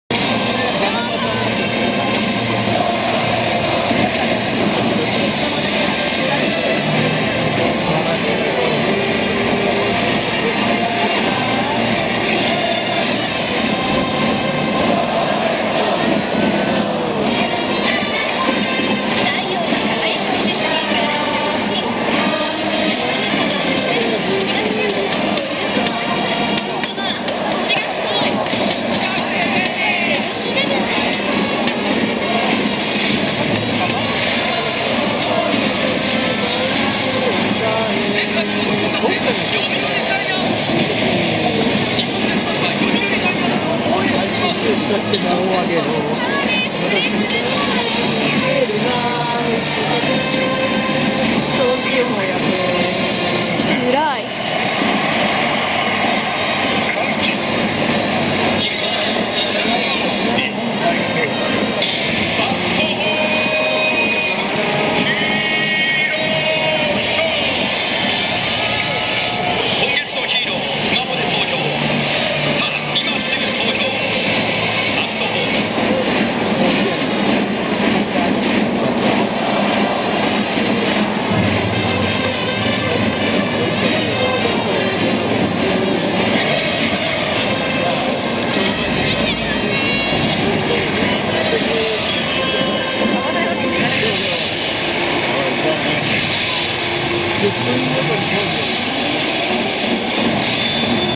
20170805 踊るドアラ真似するジャビット スタメン応援歌1−9
撮影：20170805　＠東京ドーム